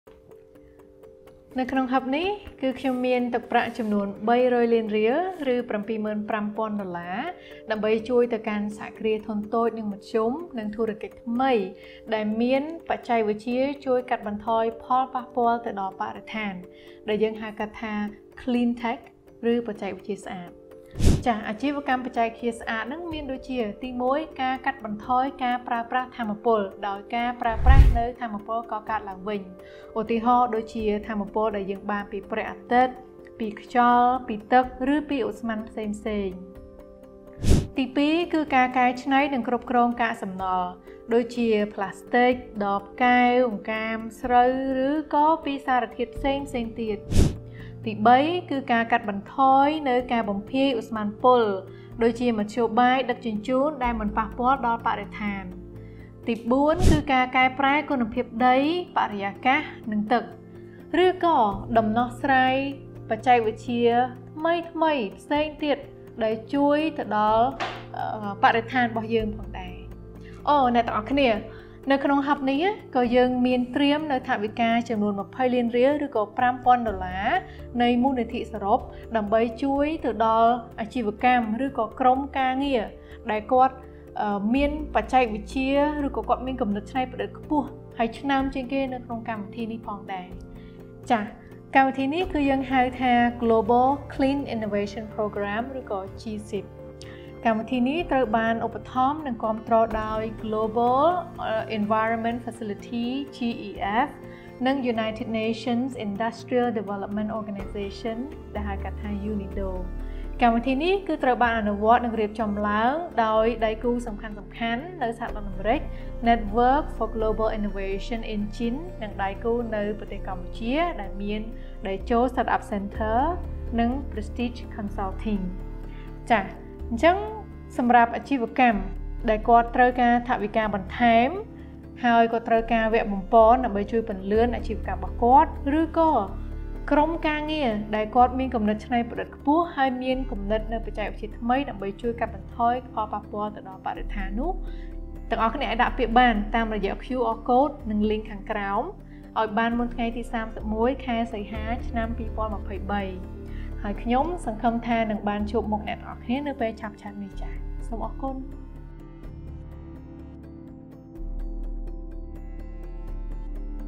វិដេអូពន្យល់ខ្លីអំពី កម្មវិធីពន្លឿនអាជីវកម្ម GCIP Cambodia រដូវកាលទី១
[English Below]
⚡️ រួសរាន់ឡើង ដាក់ពាក្យអោយបានមុនថ្ងៃទី ៣១ ខែ សីហា ដើម្បីទទួលបានប្រាក់រង្វាន់ជំនួយថវិកាយ៉ាងច្រើននិងជំនួយបច្ចេកទេស សម្រាប់សហគ្រិនដែលមានដំណោះស្រាយបច្ចេកវិទ្យាស្អាត តាមរយៈកម្មវិធីពន្លឿនអាជីវកម្ម GCIP Cambodia រដូវកាលទី១

👉 សូមទស្សនាវិដេអូពន្យល់ខ្លីរបស់លោកស្រី ស៊ូ ហ្សូលីដា អគ្គនាយិកាគ្រប់គ្រងរបស់ក្រុមហ៊ុនប្រឹក្សាយោបល់ Prestige។

គួរបញ្ជាក់ផងដែរថា កម្មវិធីនេះត្រូវបានសម្រិតសម្រាំង និងរៀបចំយ៉ាងម្ម៉ត់ចត់ពីដៃគូពាក់ពន្ធ័ជាច្រើន ទាំងក្នុងនិងក្រៅប្រទេស។ កម្មវិធីនេះគាំទ្រដោយអង្គការសហប្រជាជាតិដើម្បីអភិវឌ្ឍន៍ឧស្សាហកម្ម (UNIDO) និងមូលនិធិបរិស្ថានសកល (GEF) រួមសហការដោយ Network for Global Innovations (NGIN) ដែលសុទ្ធតែជាស្ថាប័នកំពូលក្នុងវិស័យបរិស្ថាន និងមានសហការរៀបចំដោយមជ្ឈមណ្ឌលបណ្ដុះធុរកិច្ចថ្មី “តេជោ” និង ក្រុមហ៊ុនប្រឹក្សាយោបល់ Prestige ដែលជាស្ថាប័នមានបទពិសោធន៍ក្នុងការបណ្តុះធុរកិច្ចថ្មី និងប្រឹក្សាយោបល់។

🔥 កុំអោយឱកាសដ៏មានតម្លៃនេះរំលងផុតទៅ ចូលរួមជាមួយGCIP Cambodia នាំកម្ពុជាឆ្ពោះទៅរកទំនើបកម្មក្នុងវិស័យបច្ចេកវិទ្យាស្អាត

GCIPCAMBODIA

✨ Apply now, before 31 August to have a chance of winning the grant award and technical support to scale your cleantech venture through the GCIP Cambodia Accelerator Program Cohort 1.

👉 Watch this short video featuring Mrs. Jolyda Sou, Managing Director of Prestige Consulting to learn more about the program.

It should also be noted that this program is an initiative established and supported by the United Nations Industrial Development Organizations (UNIDO) and Global Environment Facility (GEF), collaborated with Network for Global Innovations (NGIN), all of which are prominent and leading entities in the environmental industry. And, co-organized by Techo Startup Center and Prestige Consulting.
Don’t let this opportunity slip away, join GCIP Cambodia in leading Cambodia towards modernization in cleantech.

Application deadline: August 31, 2023 (11:59 p.m.)